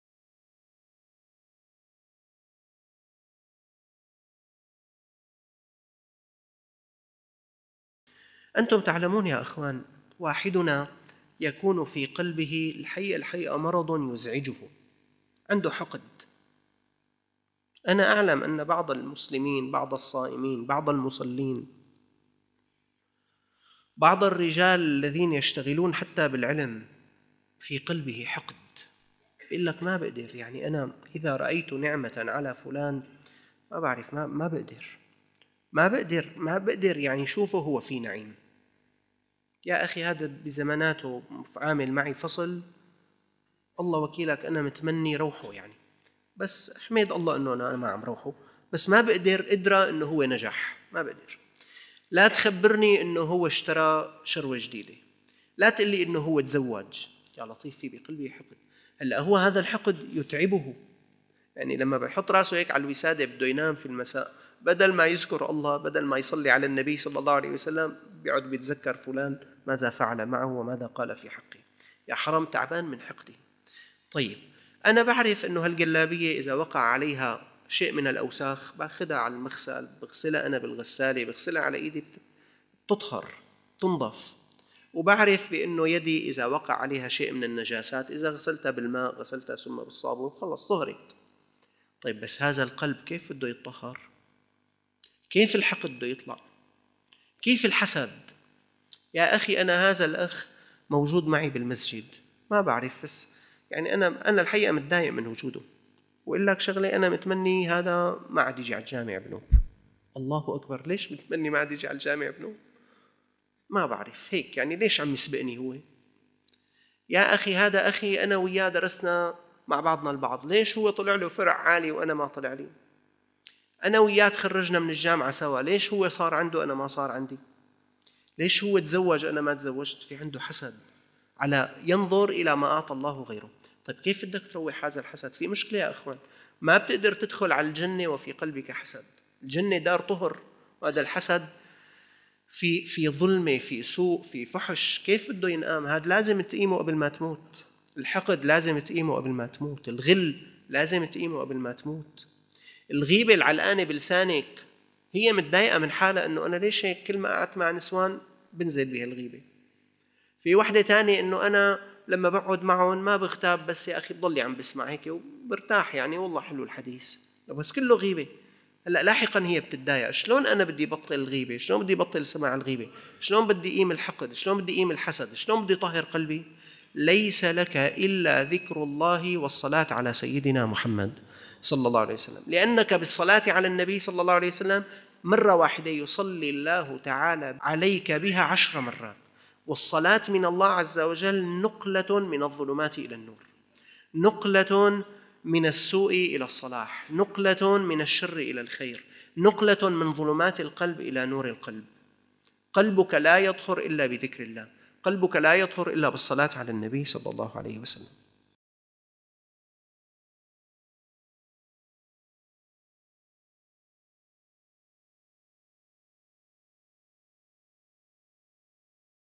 بذلك تُطهِّر قلبَك - د.محمد خير الشعال
قال رسول الله صلى الله عليه وسلم: «مَنْ صَلَّى عَلَيَّ صَلَاةً صَلَّى اللهُ عَلَيْهِ بِهَا عَشْرًا» [مسلم].
قال تعالى:{هُوَ الَّذِي يُصَلِّي عَلَيْكُمْ وَمَلَائِكَتُهُ لِيُخْرِجَكُم مِّنَ الظُّلُمَاتِ إِلَى النُّورِ ۚ وَكَانَ بِالْمُؤْمِنِينَ رَحِيمًا}.